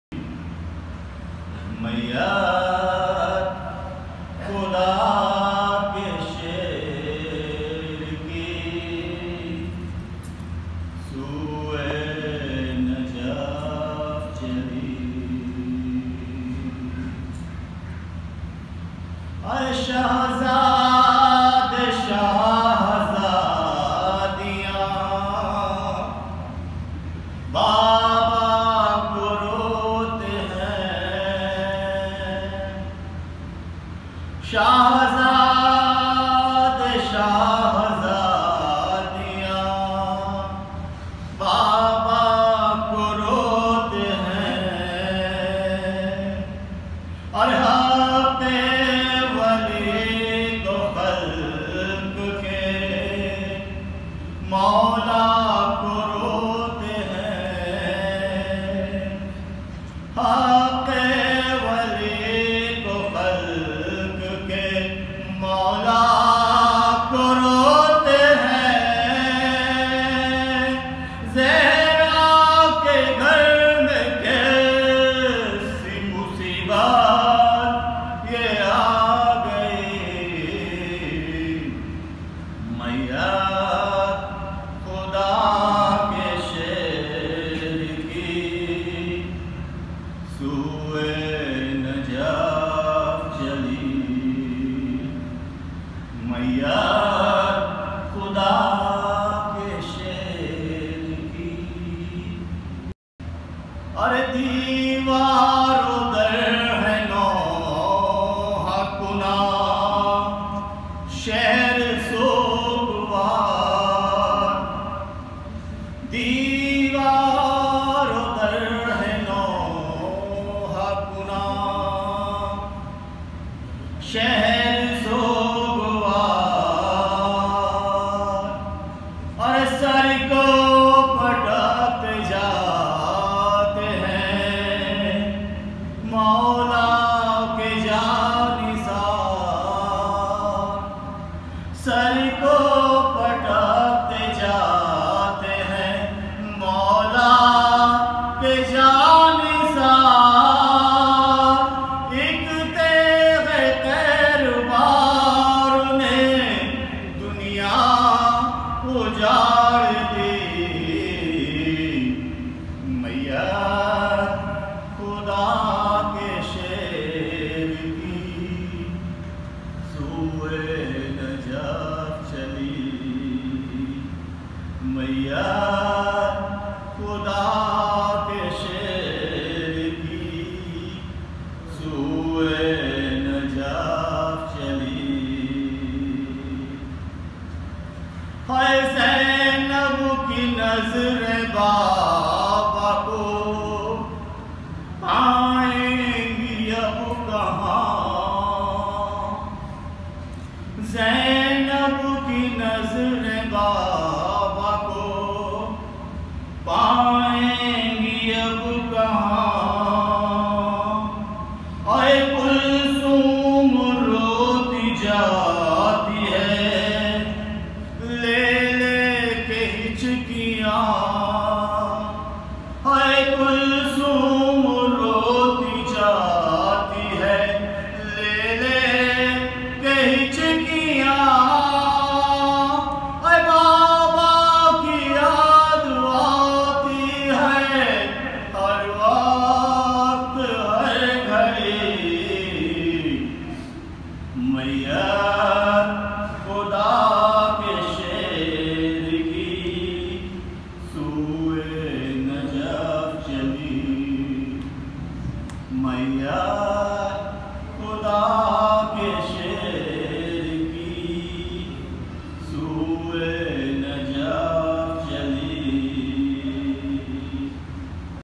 Marsiya 2019 | 21 Ramzan Imambara Pune 27 May 2019
21 Ramzan -27 May 2019
Place: Hmji Irani Imambara Pune
shabe e Shahadate Maula Ali a.s